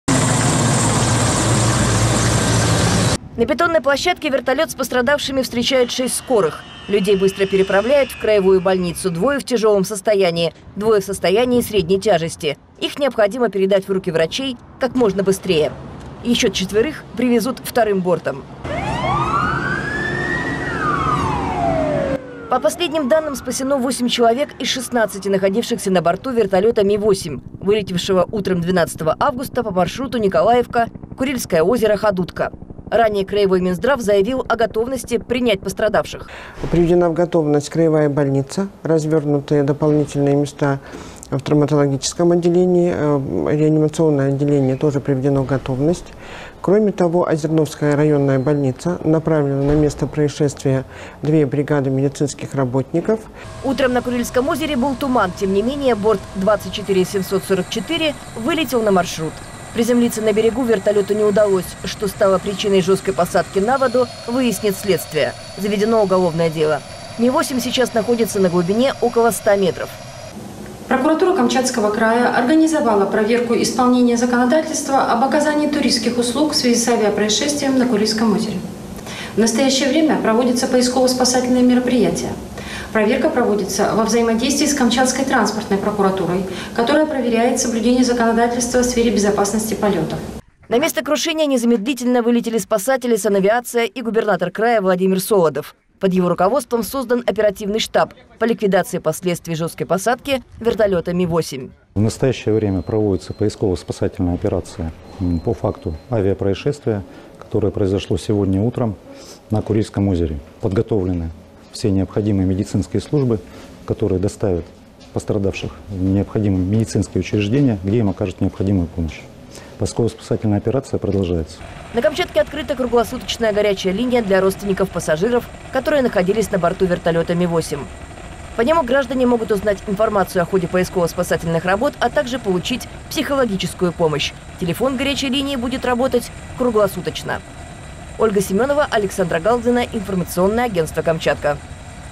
Выживших при падении вертолета доставили в Петропавловск
Пострадавших в результате жесткой посадки вертолёта МИ-8 на Курильское озеро доставили в Петропавловск-Камчатский.